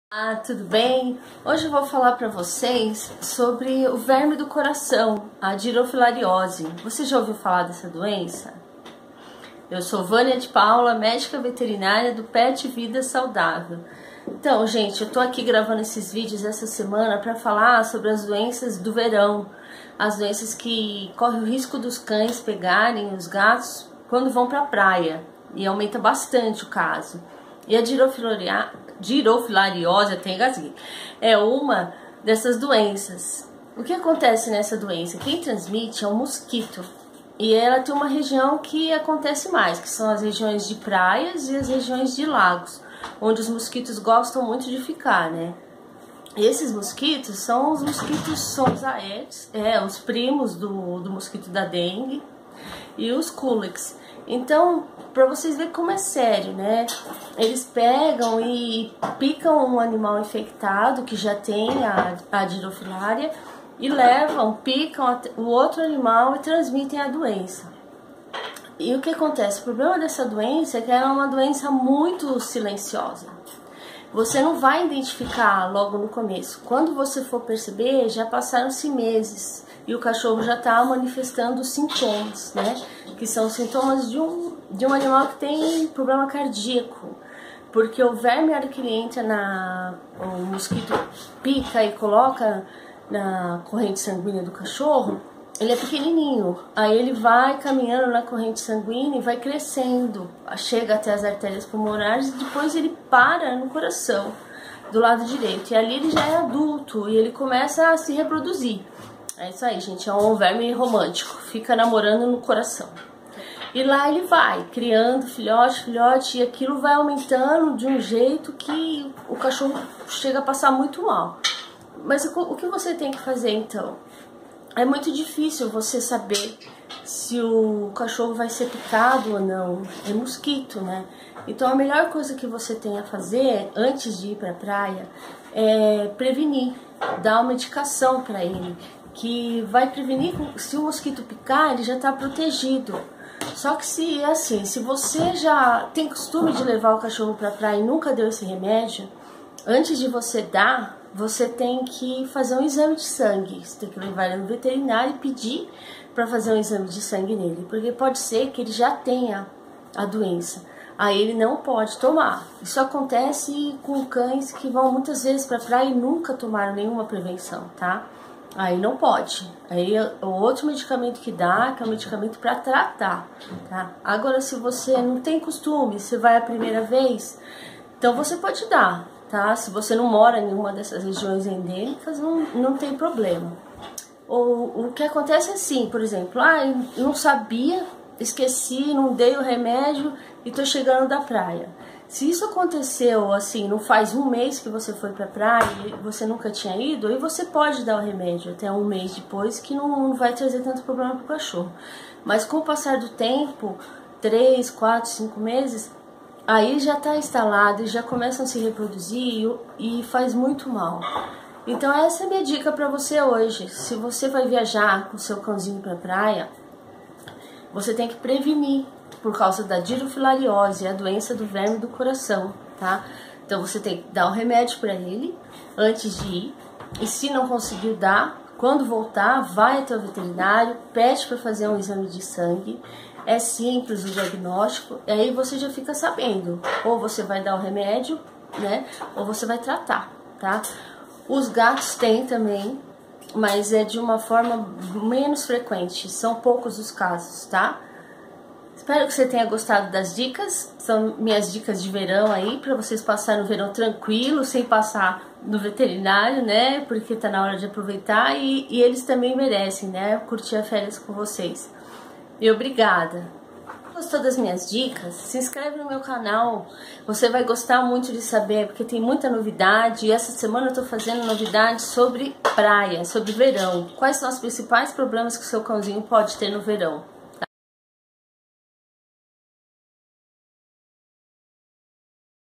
Verme do Coração Como Proteger Meu Cachorro
Aprenda a escolher sozinho qual a melhor ração para o seu cachorro e seu gato

Neste vídeo te ensino como proteger seu cachorro do verme do coração que causa a Dirofilariose
Não leve seu cachorro para praia antes de  dar a medicação para ele
Prevenir é melhor que remediar
doenças do verão
verme do coração em cachorros
tosse de cachorro
cachorro doente